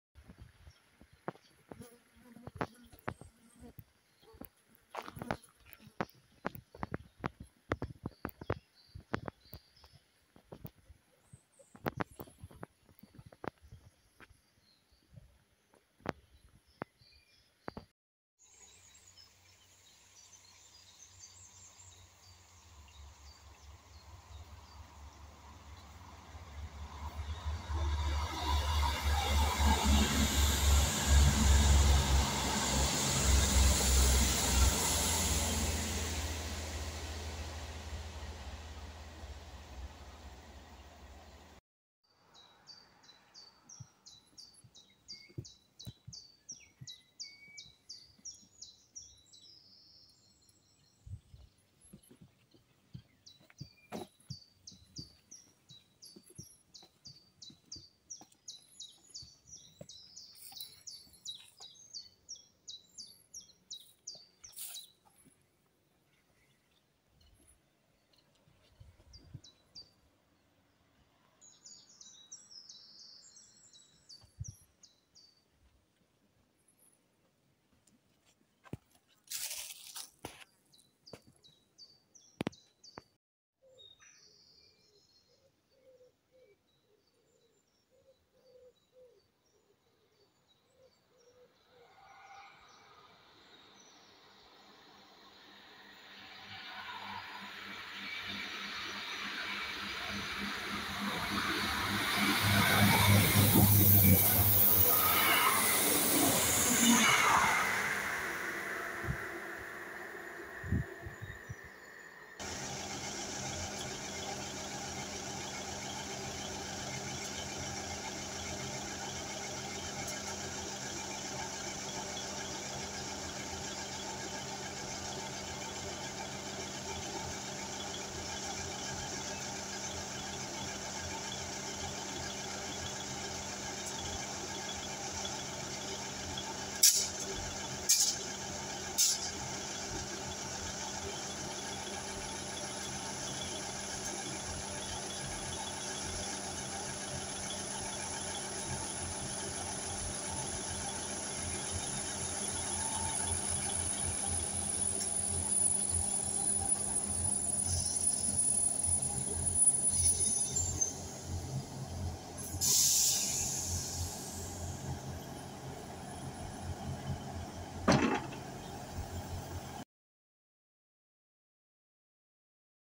Class 159's at various locations around Bugley & Gillingham Dorset 22nd June 23.
Various locations and units were captured around the Bugley and Gillingham area. Video taken on the 22nd of June 2023.